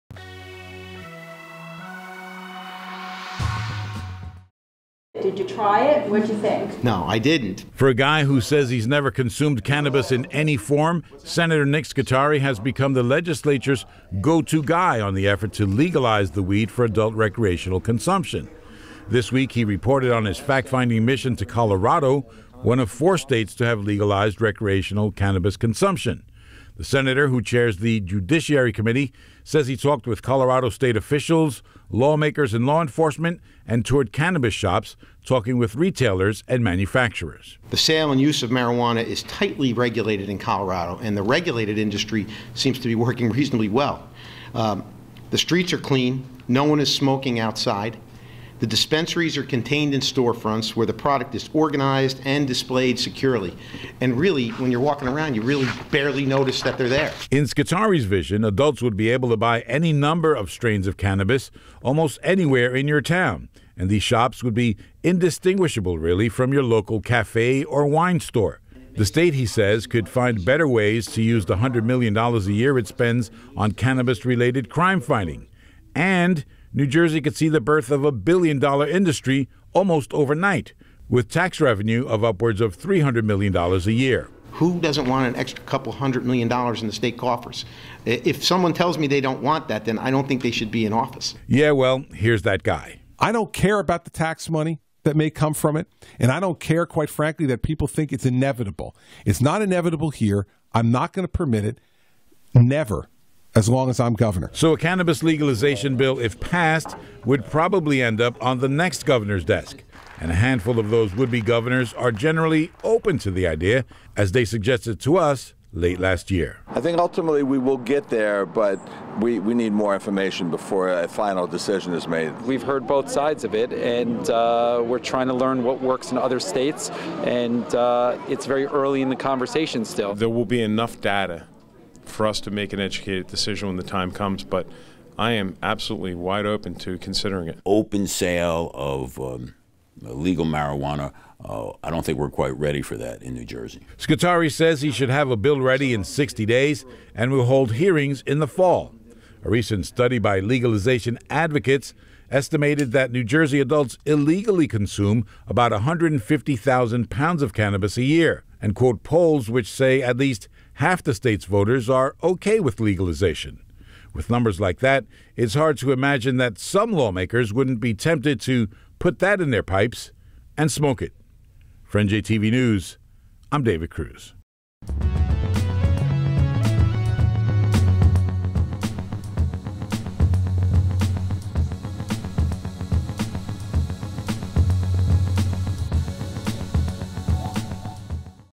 Cannabis Legalization Is Not a Pipe Dream, Says Sen. Scutari
Sen. Nick Scutari visited Colorado to assess how that state is dealing with legalized recreational marijuana use. He hopes to legalize it in New Jersey.